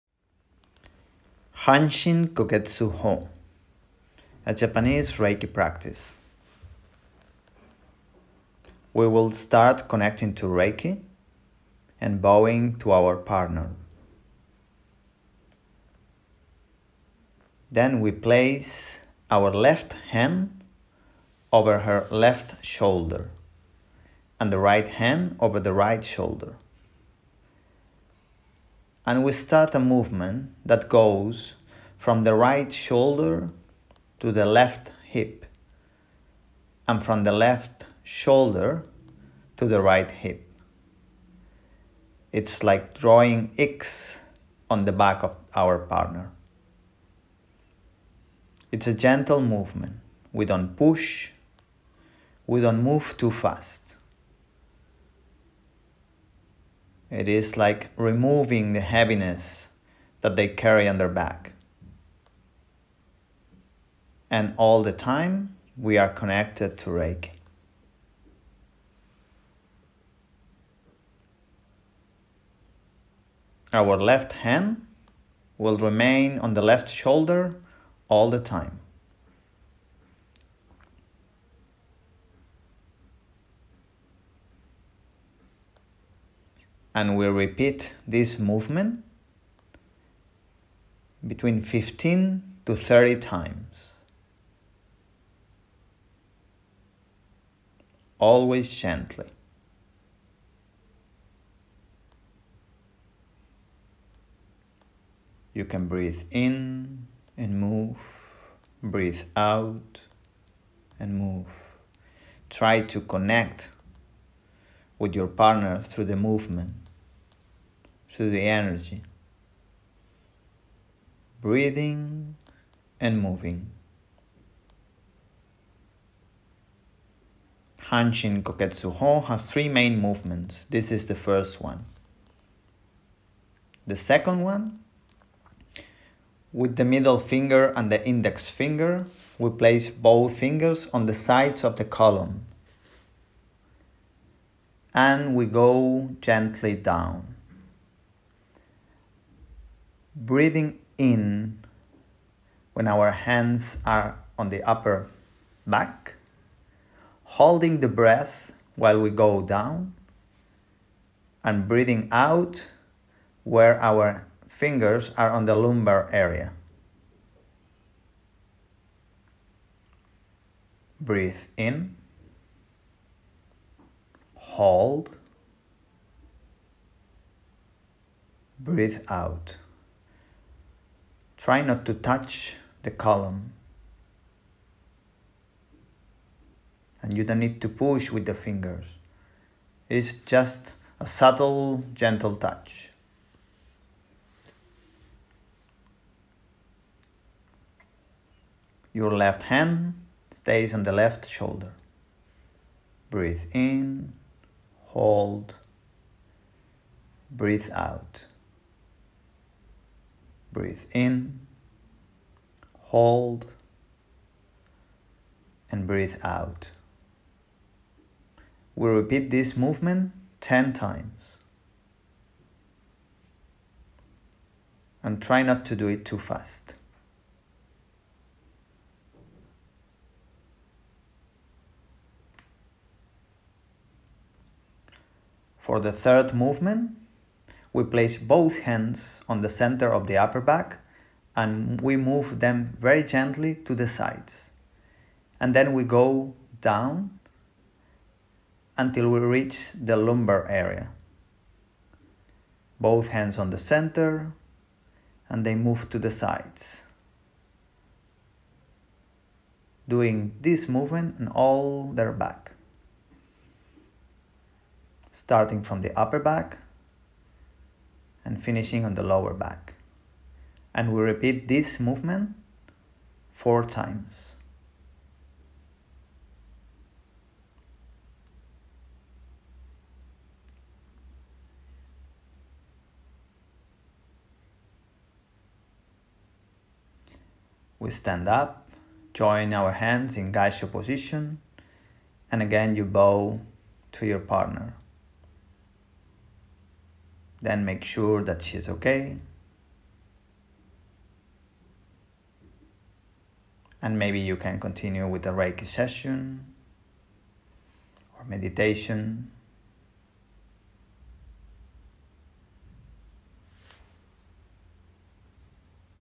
Reiki: Hanshin Koketsu ho (Reiki)
Hanshin koketsu ho, Japanese Reiki technique.  This Japanese practice is used to treat the patient's back.

Hanshin Koketsu ho has 3 main movements: The first one drawing several X on the patient’s back, the second one moving down the index and the middle fingers on both sides of the spine, and the third one moving both hands from the center of the back to the sides —going from the cervical area to the lumbar area.